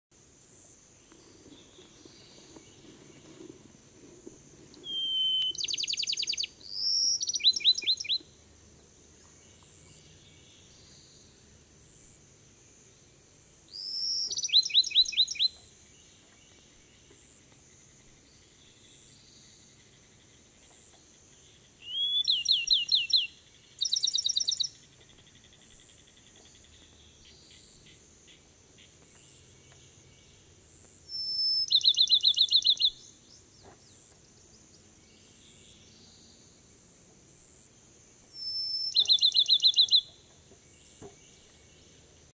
Bachman's Sparrow Singing
Bachman's Sparrow, Francis Marion National Forest, Charleston County, SC, 6/25/2016